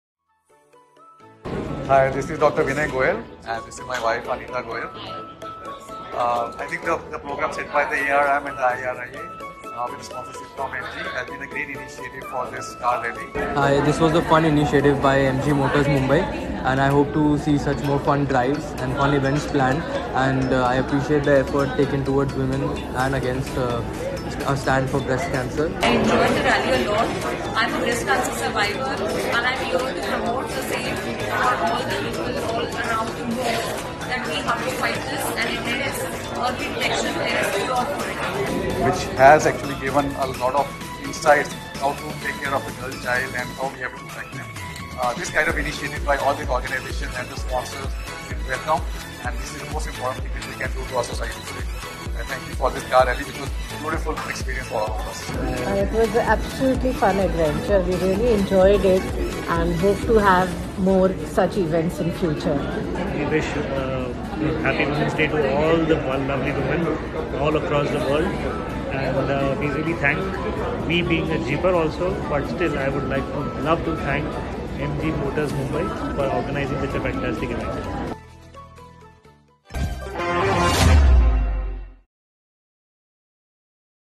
MG Rally Women's Day - Testimonials
We would like to express our gratitude towards Radiologists and MG patrons for their dynamic participation in the drive.

Here are some invigorating moments which they want to share.